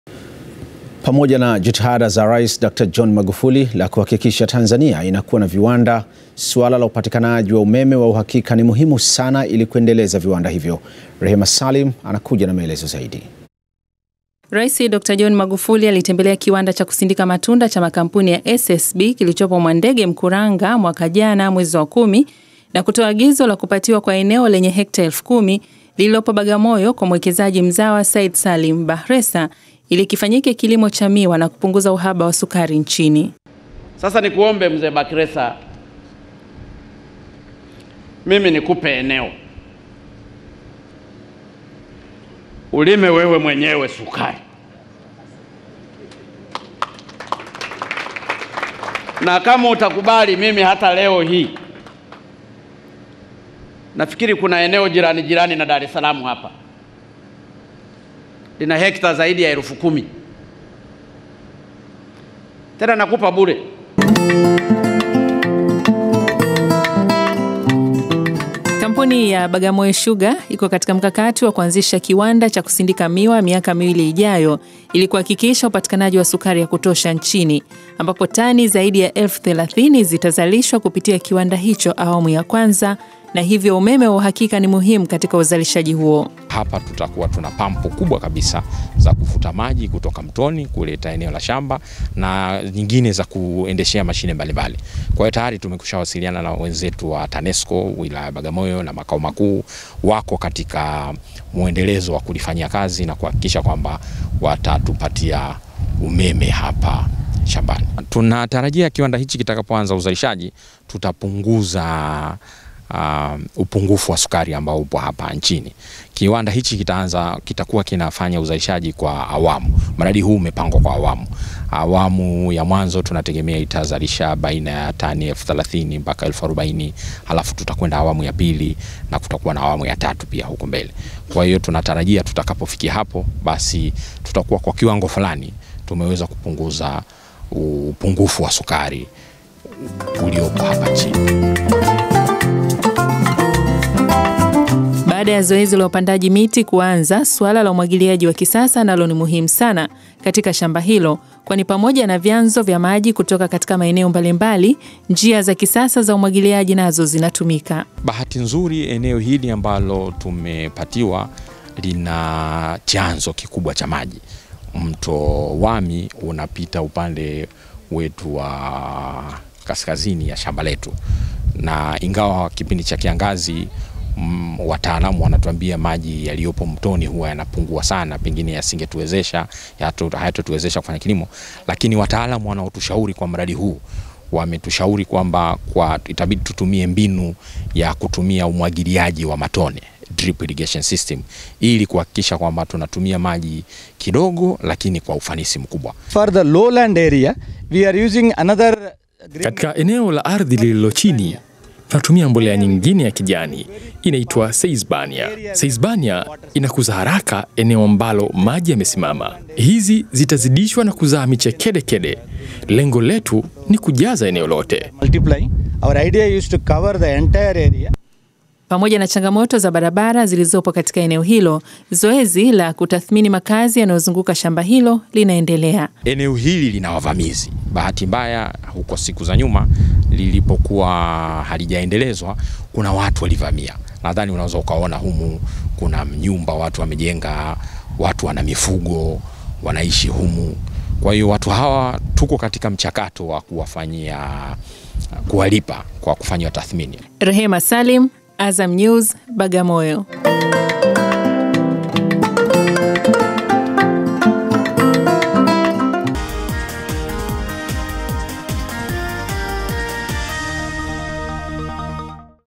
Azam TV - Bakhresa kujenga kiwanda cha sukari Bagamoyo ifikapo 2020
Kampuni ya Bagamoyo Sugar ya mwekezaji mzawa Said Salim Bakhresa imetangaza mpango wa kujenga kiwanda kikubwa cha sukari wilayani Bagamoyo ili kukomesha upungufu wa bidhaa hiyo nchini.